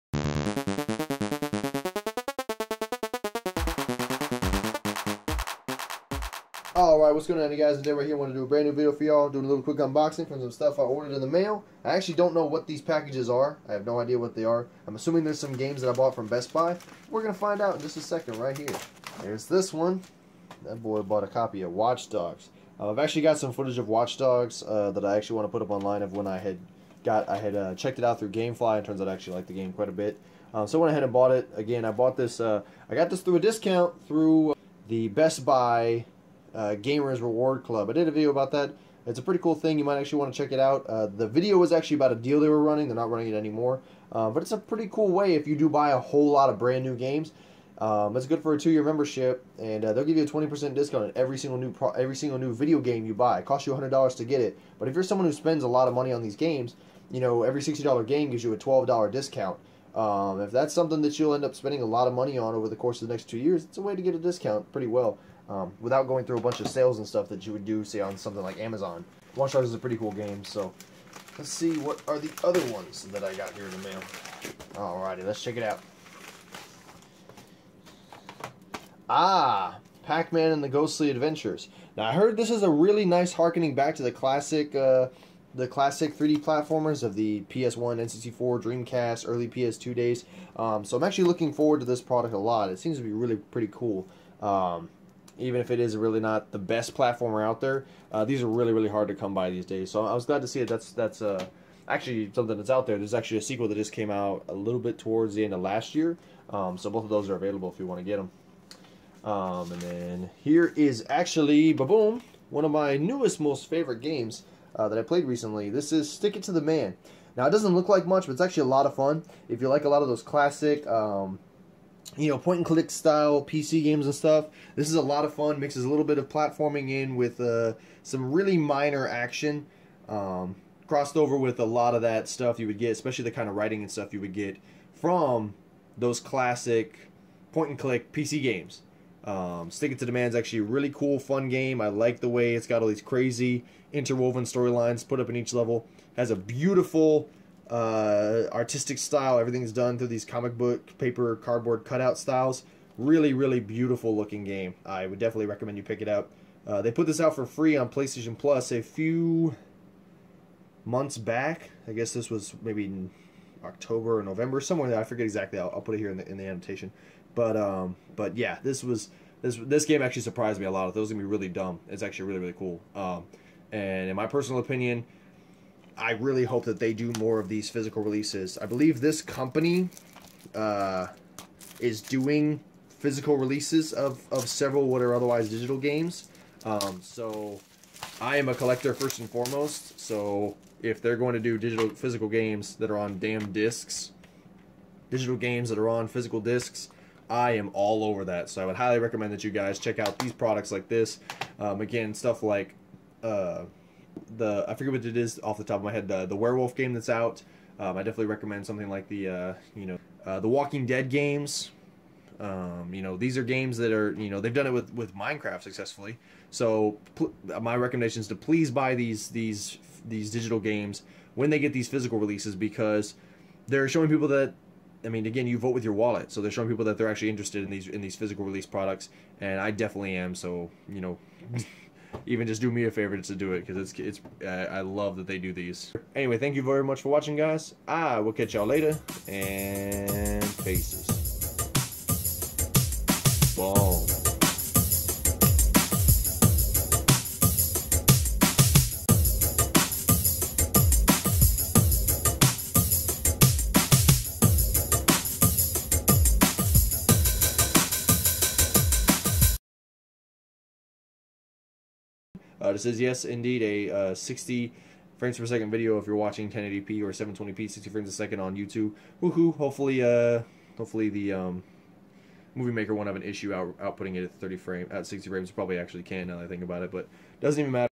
Unboxing 3 New Games!
In this video I'm unboxing 3 new games I got in the mail on discount through the BestBuy Gamer's Club Unlocked membership.
Watch Dogs for the PS4, Pac-Man and the Ghostly Adventures for the Xbox 360, and lastly Stick it to the Man for PS4!